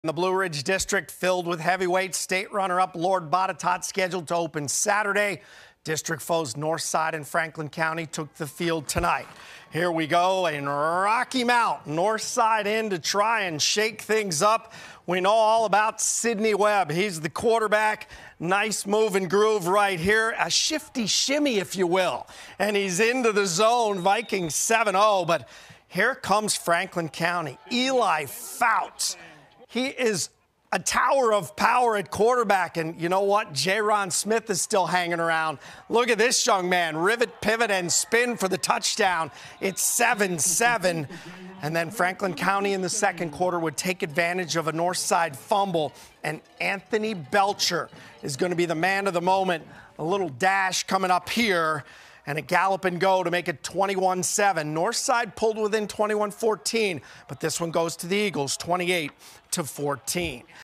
Franklin County vs. Northside
Franklin County scored 21 unanswered points to beat the Northside Vikings on Thursday night, 28-14.